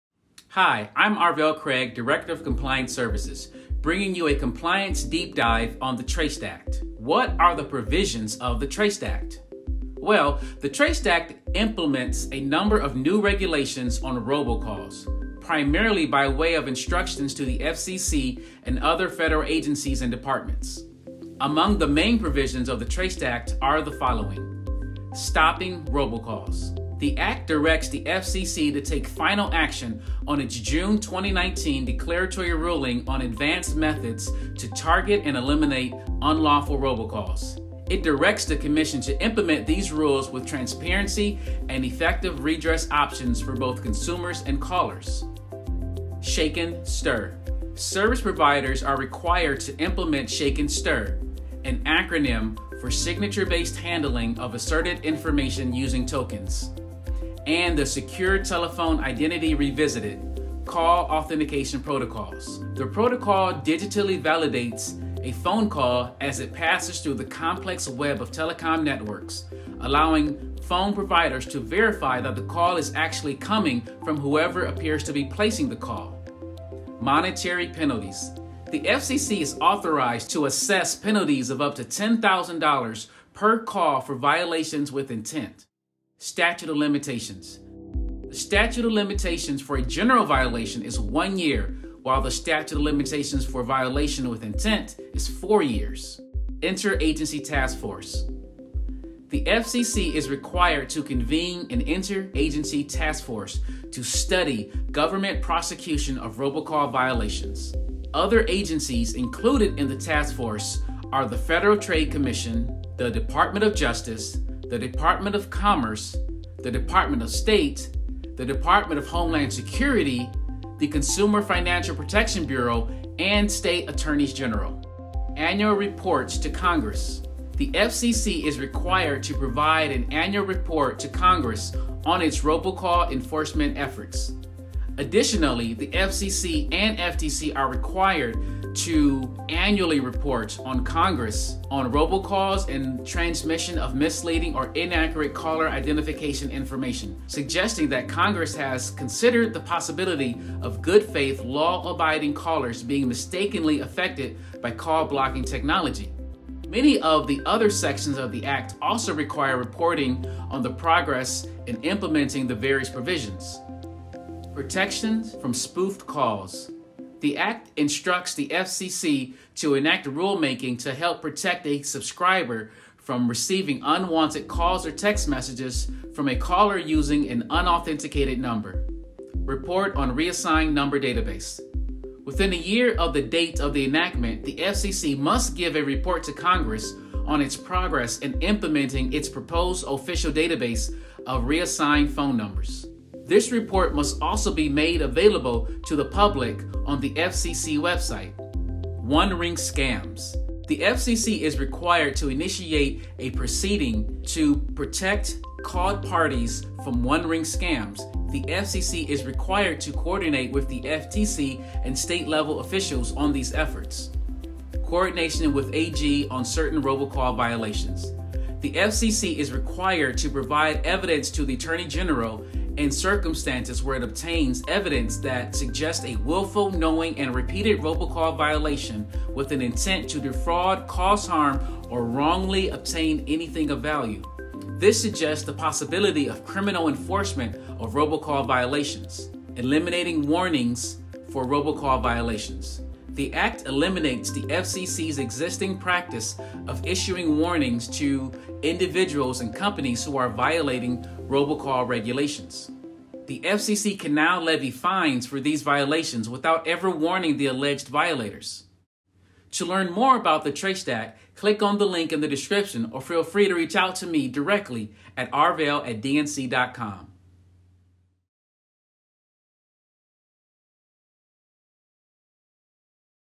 What are the provisions of the TRACED Act?
The TRACED Act implements a number of new regulations on robocalls, primarily by way of instructions to the FCC and other federal agencies and departments. Among the main provisions of the TRACED Act are the following:

Stopping Robocalls
The Act directs the FCC to take final action on its June 2019 Declaratory Ruling on Advanced Methods To Target and Eliminate Unlawful Robocalls. It directs the commission to implement these rules “with transparency and effective redress options for both… consumers; and… callers.”

SHAKEN/STIR
Service providers are required to implement SHAKEN/STIR—an acronym for Signature-based Handling of Asserted Information Using toKENs (SHAKEN) and the Secure Telephone Identity Revisited (STIR)—call authentication protocols. The protocol digitally validates a phone call as it passes through the complex web of telecom networks, allowing phone providers to verify that the call is actually coming from whoever appears to be placing the call.

Monetary Penalties
The FCC is authorized to assess penalties of up to $10,000 per call for violation with intent.

Statute of Limitations
The statute of limitations for a general violation is one year while the statute of limitations for violation with intent is four years.

Interagency Task Force
The FCC is required to convene an interagency task force to study government prosecution of robocall violations. The other agencies included in the task force are the Federal Trade Commission (FTC), Department of Justice (DOJ), Department of Commerce (DOC), Department of State (DOS), Department of Homeland Security (DHS), Consumer Financial Protection Bureau (CFPB), and state attorneys general.

Annual Reports to Congress
The FCC is required to provide an annual report to Congress on its robocall enforcement efforts. Additionally, the FCC and FTC are required to annually report to Congress on “Robocalls and Transmission of Misleading or Inaccurate Caller Identification Information”—suggesting that Congress has considered the possibility of good faith, law abiding callers being mistakenly affected by call-blocking technology. Many of the other sections of the Act also require reporting on the progress in implementing the various provisions.

Protections from Spoofed Calls
The Act instructs the FCC to enact a rulemaking to “help protect a subscriber from receiving unwanted calls or text messages from a caller using an unauthenticated number.”

Report on Reassigned Number Database
Within a year of the date of enactment, the FCC must give a report to Congress on its progress in implementing its proposed official database of reassigned phone numbers. This report must also be made available to the public on the FCC website.

Protection from One-Ring Scams
The FCC is required to “initiate a proceeding to protect called parties from one-ring scams.” The FCC is required to coordinate with the FTC and state-level officials on these efforts.

Coordination with AG on Certain Robocall Violations
The FCC is required to provide evidence to the Attorney General in circumstances where it “obtains evidence that suggest a willful, knowing, and repeated robocall violation with an intent to defraud, cause harm, or wrongly obtain anything of value.” This suggests the possibility of criminal enforcement of robocall violations.

Eliminating Warnings for Robocall Violations
The act eliminates the FCC's existing practice of issuing warnings to individuals and companies who are violating robocall regulations. The FCC can now levy fines for those violations without ever warning the alleged violators.